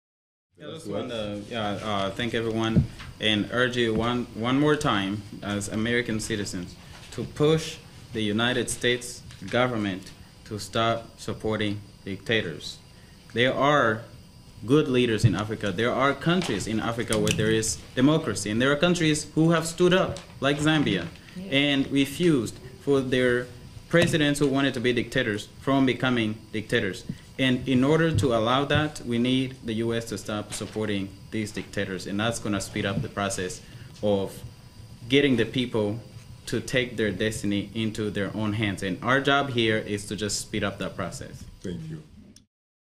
Mr. Claude Gatebuke's Closing Remarks during the African Leaders Summit Call For End To Strong Men
Matthew 7:7 Press Latest Update
WASHINGTON, D.C - USA - National Press Club hosted an African Leaders Summit: Call For End To Support Of Strong Men on Thursday, July 31, 2014. A conversation hosted by Dr. Shaka Ssali, VOA Straight Talk Africa Host.